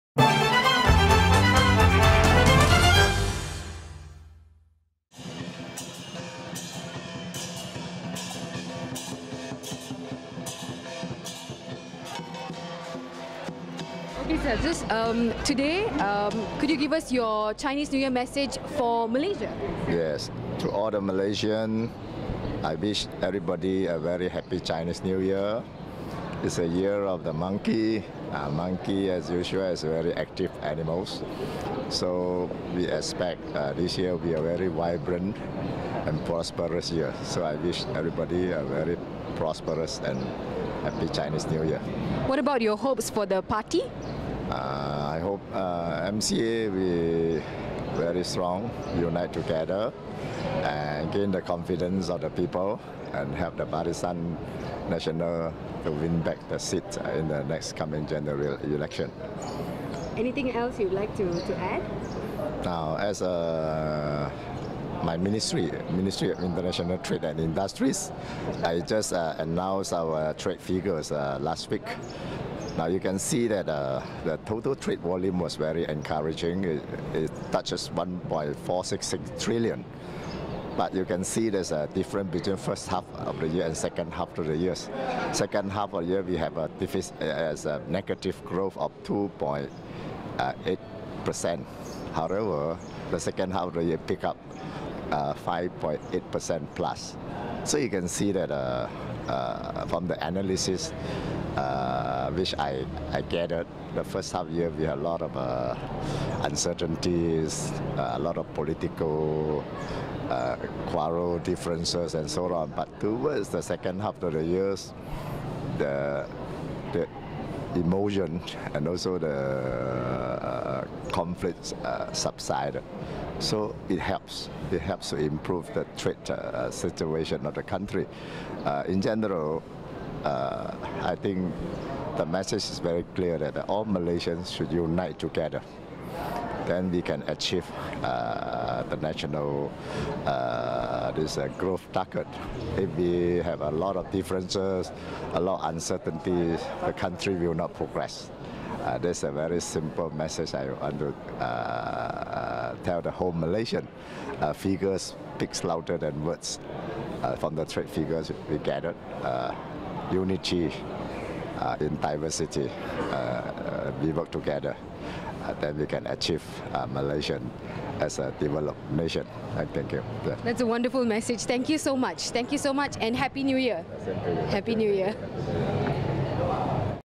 MCA CNY Greetings: Datuk Seri Ong Ka Chuan
Chinese New Year open house celebrations organised by MCA.